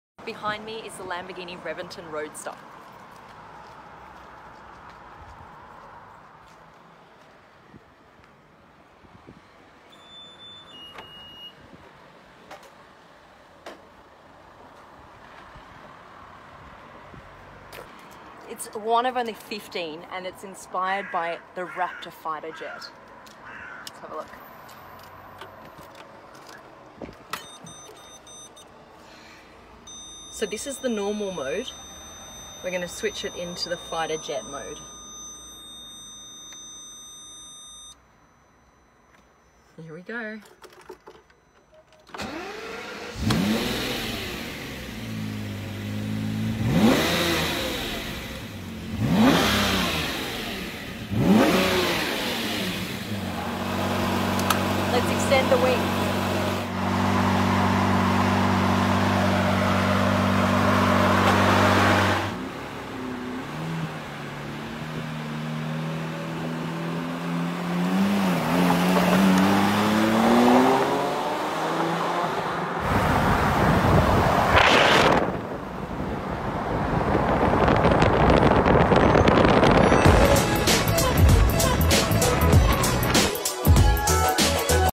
The Lamborghini Reventon Roadster
What a thrill, driving this 1 of 15 Lamborghini in London today! Looks like an Aventador in the front and a Murci in the back... thoughts??
Thanks to Joe Macari Performance Cars